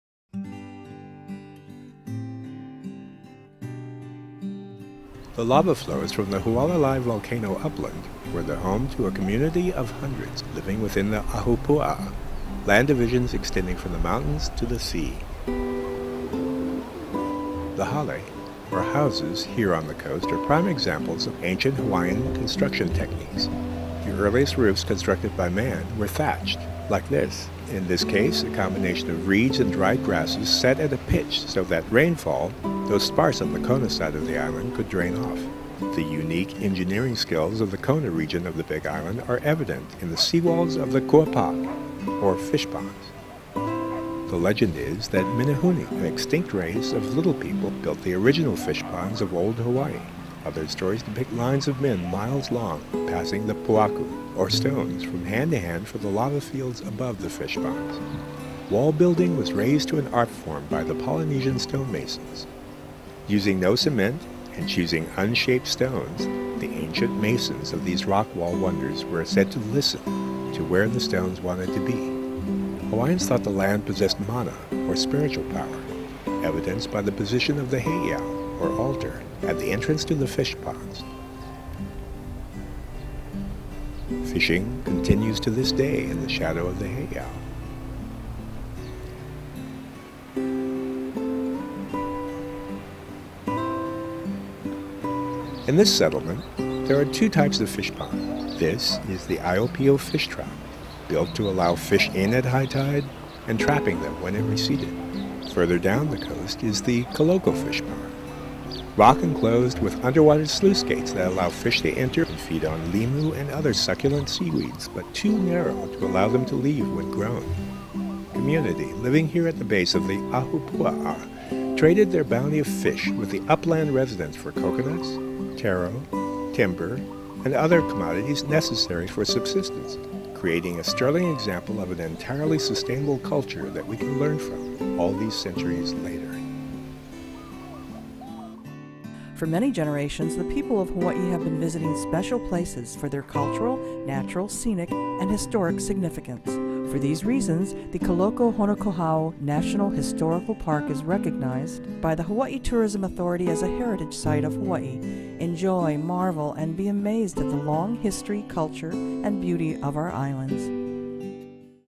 Heritage Site - Kahoko-Honokohau National Historical Park
Home to two amazing Hawaiian fishponds that show the engineering acuity of Native Hawaiians, you can see four different ahupuaa (traditional sea to mountain land divisions), as well as heiau (sacred temples) and kii pohaku (petroglyphs).